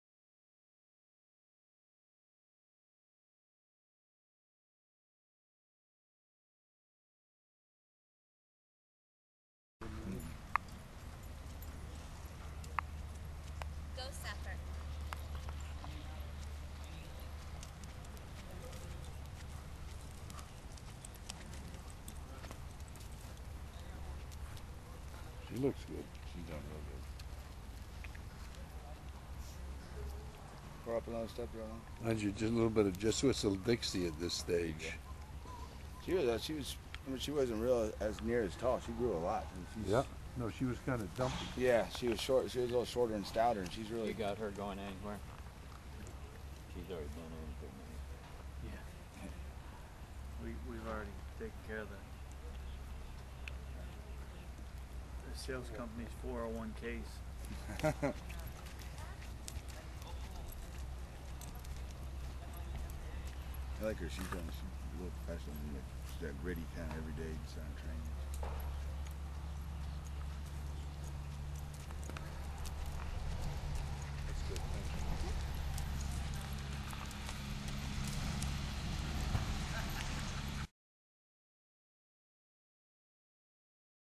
Seeking The Ghost - 1/6/09
Seeking The Ghost at David Scanlon's farm in Ocala, Florida.